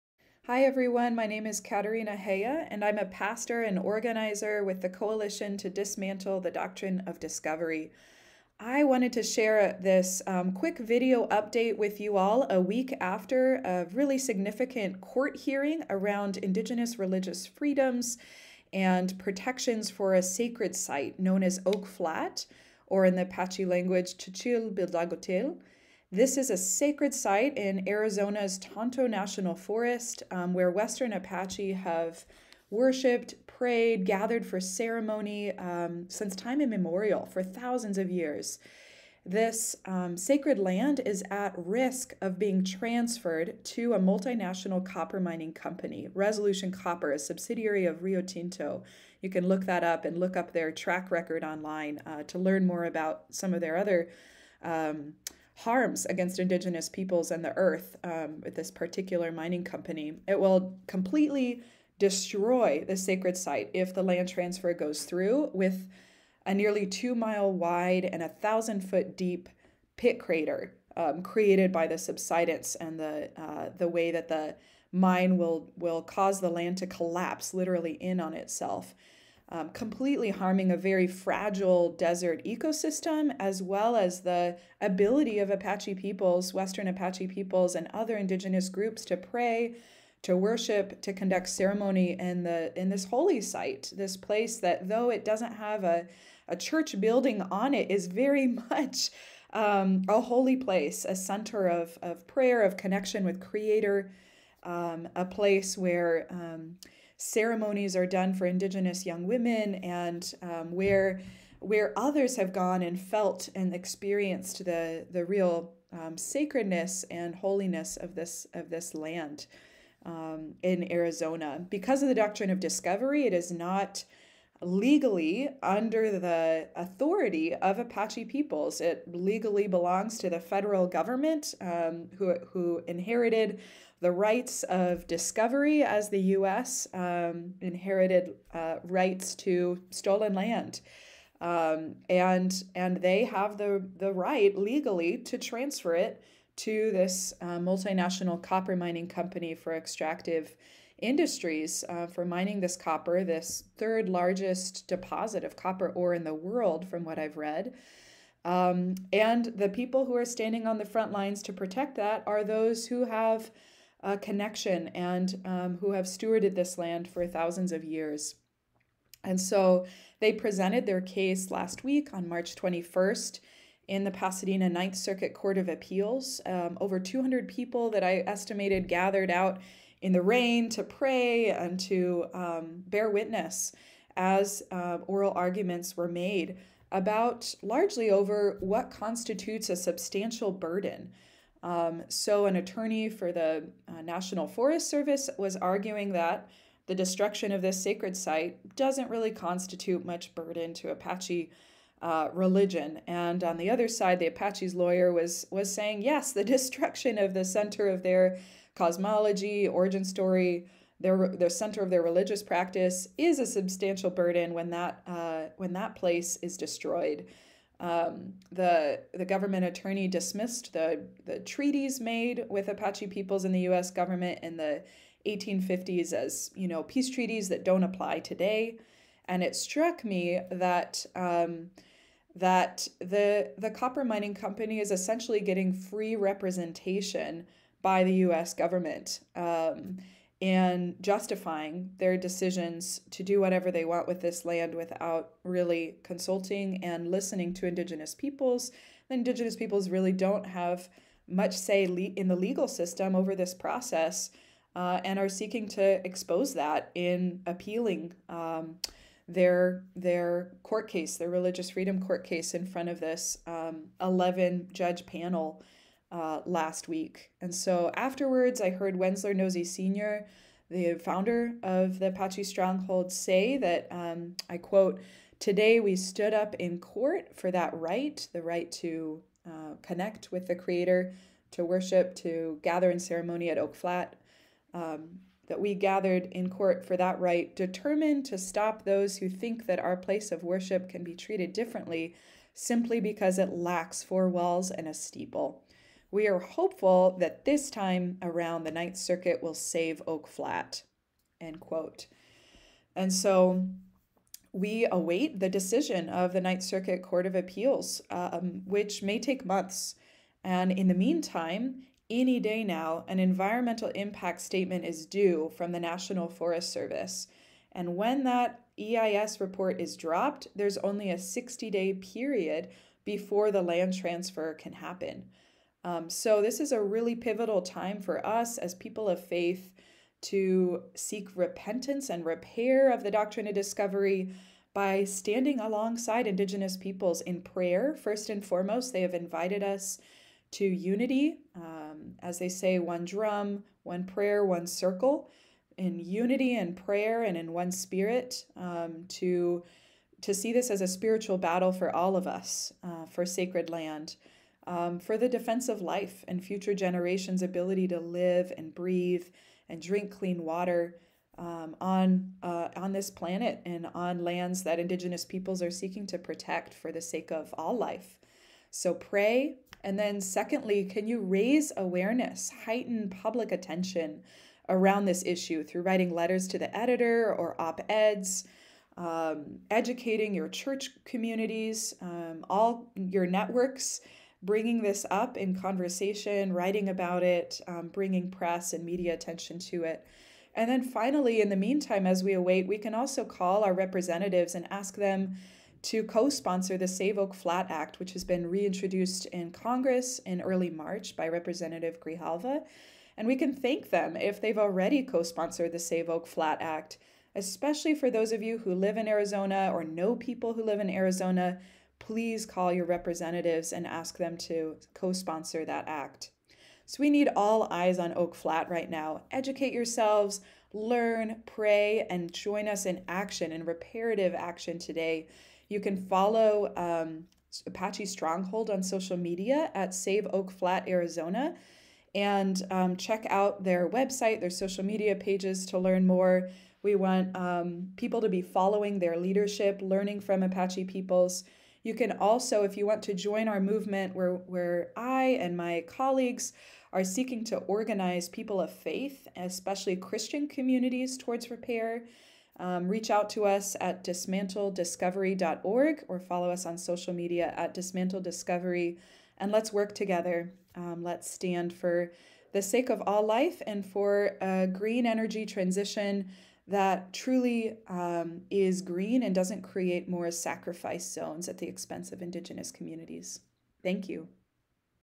What can we do to join Apache Stronghold and protect Oak Flat today?
Katerina Gea shares a short update after the March 21, 2023 Ninth Circuit Court of Appeals hearing of Apache Stronghold v. United States. NOTE: Katerina's opinions and perspectives are her own, and do not represent the Apache Stronghold. Suggested actions for support of Apache Stronghold include prayer, raising media attention about this case, and supporting the Save Oak Flat Act reintroduced in Congress.